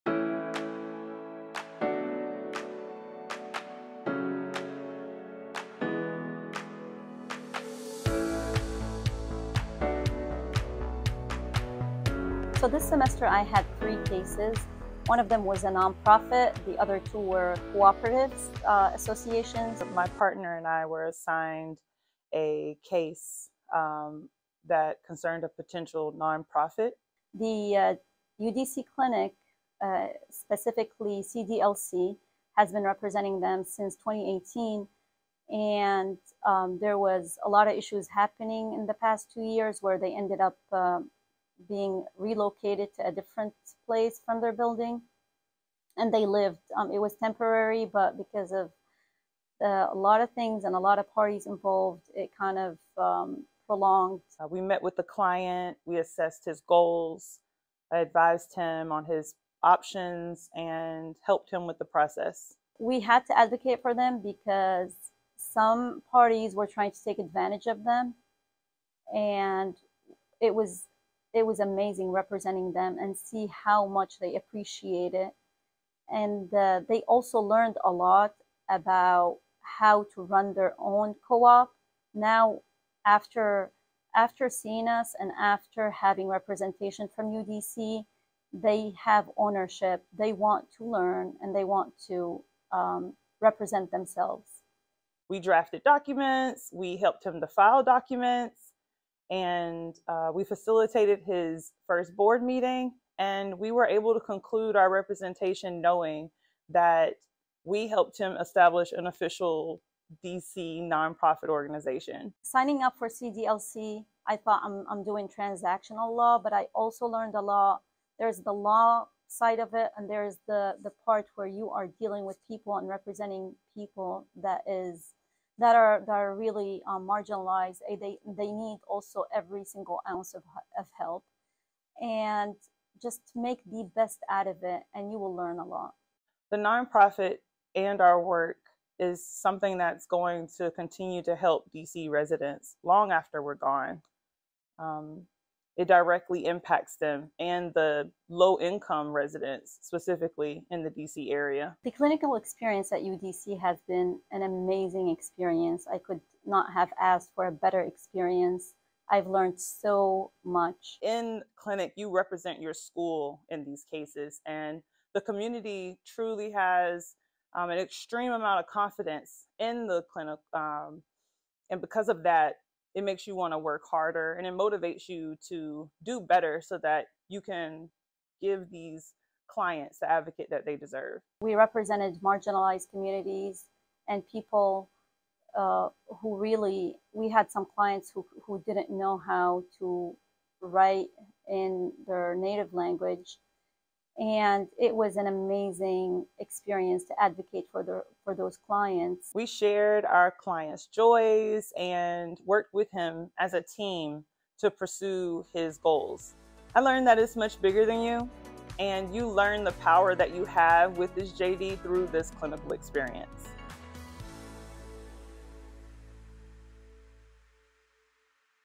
Clinic Highlights: Community Development Law Clinic
Shagaf Shehadeh '22 and Sonya Williams (3L) on the greater impact of cases they worked on in the Community Development Law Clinic